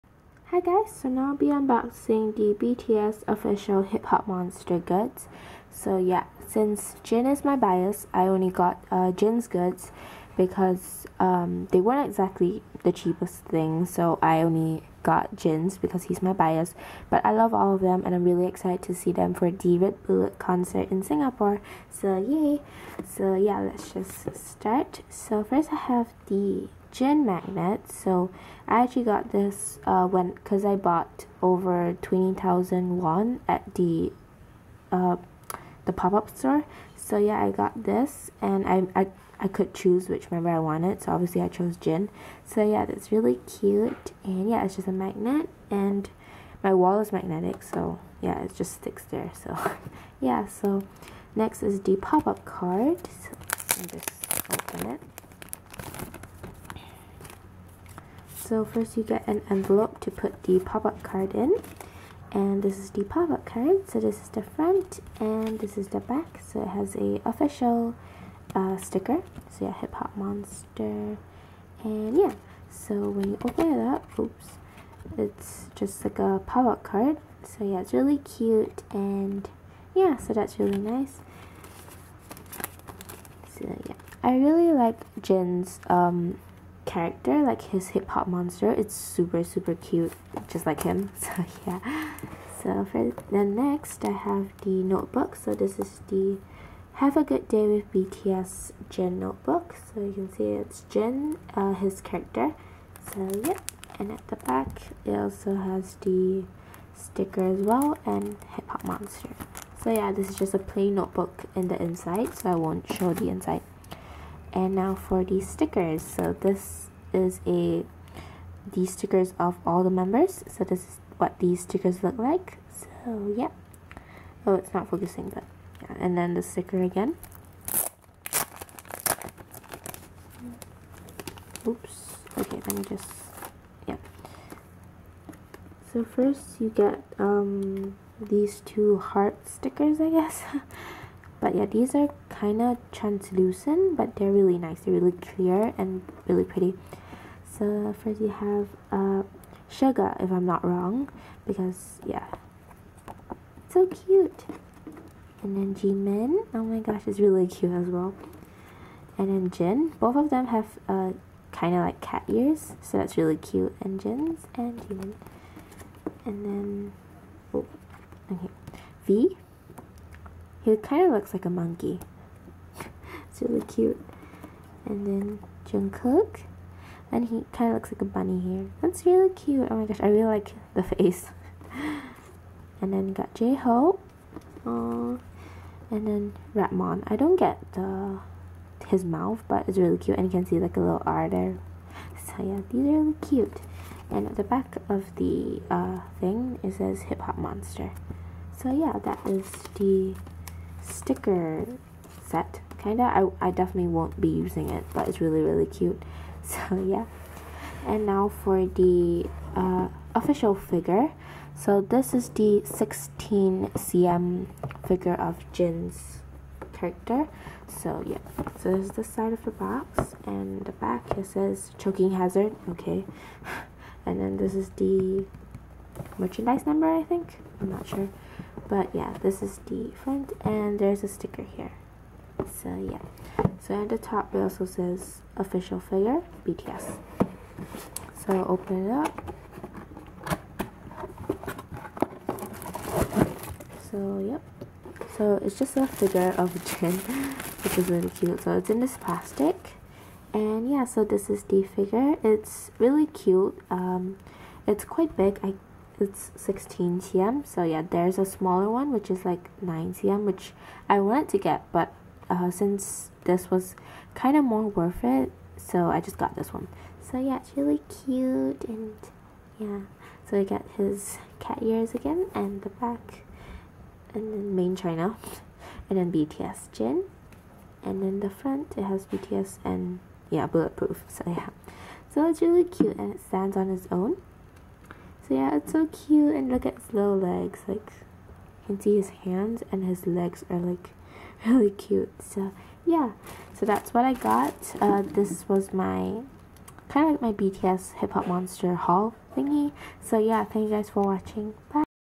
BTS / Bangtan Boys Jin Hip Hop Monster Goods Haul! ( Figure, Stickers, Pop Up Card, Notebook )
This is my mini haul from the bts pop up store in Korea!

ALSO, ITS 1 MORE DAY TO BTS TRB IN SG HI-5 EVENT AND IM SO EXCITED GDI

I CANT WAIT TO TOUCH THEIR HANDSSS HAHAH

xx